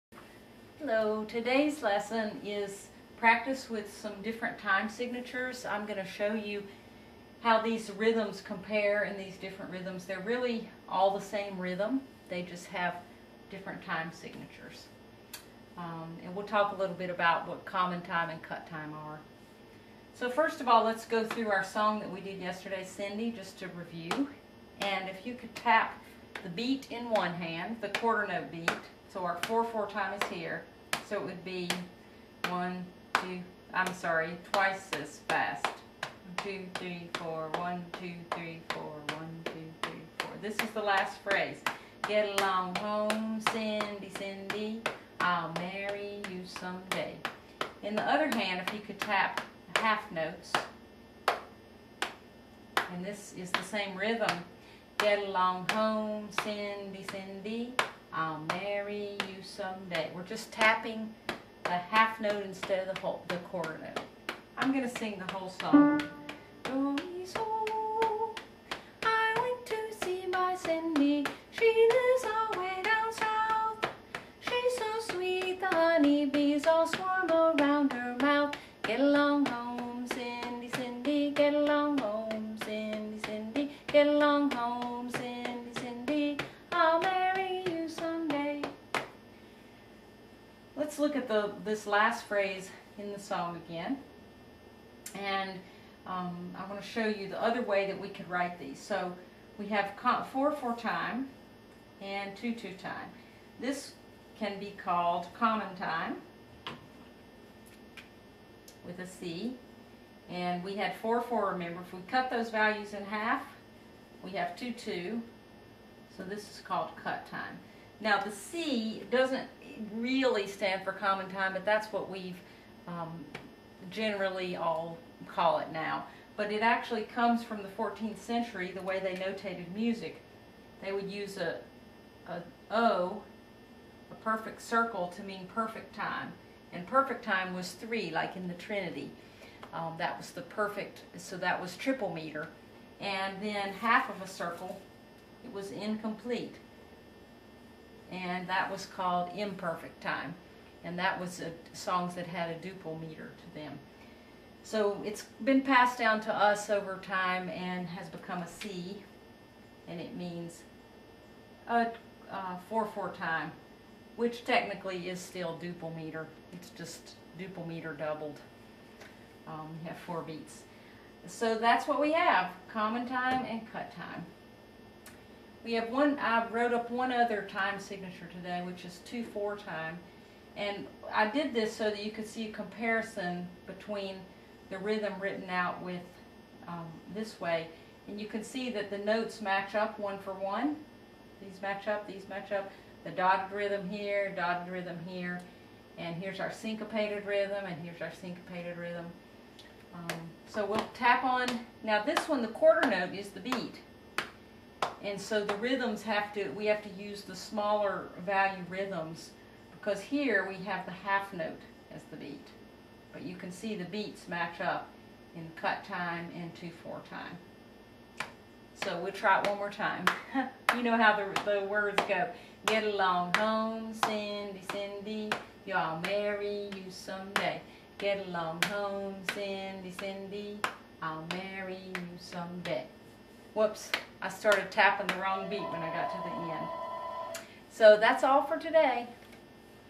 101 - common time and cut time
This lesson explains common and cut time.  It also compares the same ryythm notated in 2/4, 4/4 and 2/2.